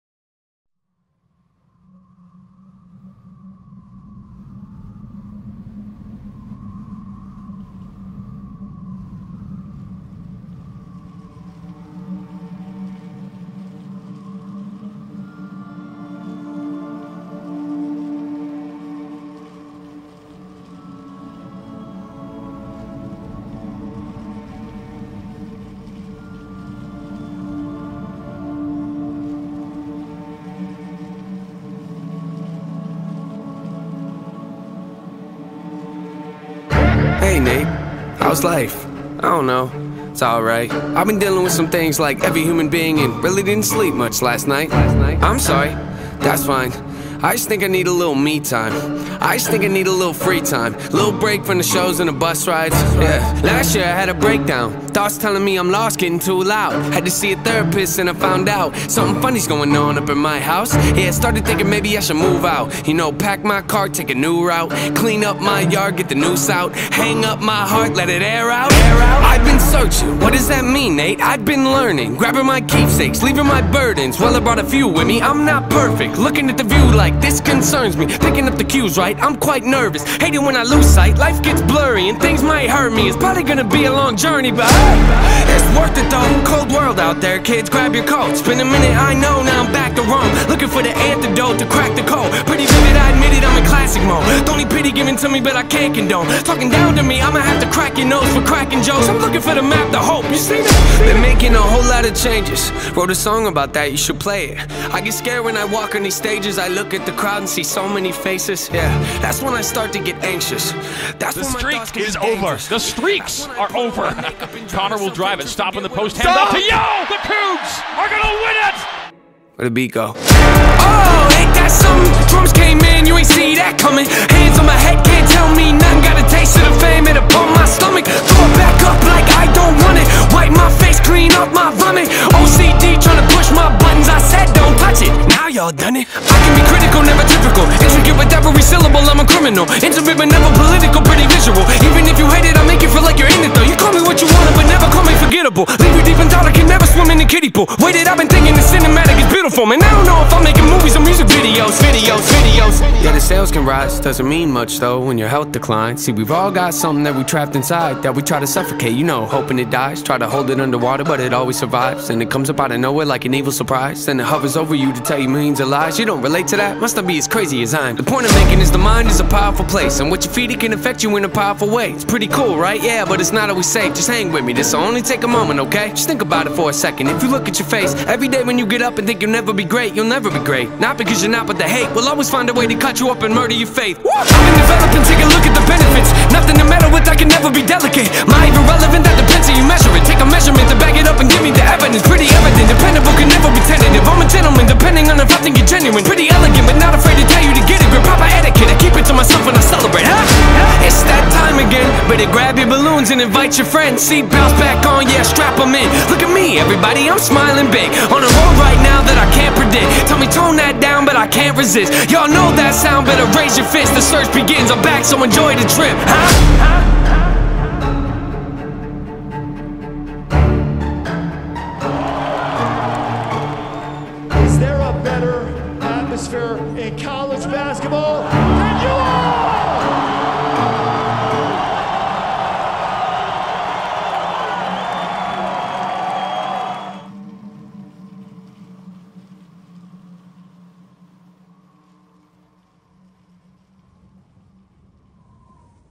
BYU Basketball Pump Up 2020
Best highlights from BYU Basketball's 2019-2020 season. Cut short to Corona, not cut short to memories. Go Cougs!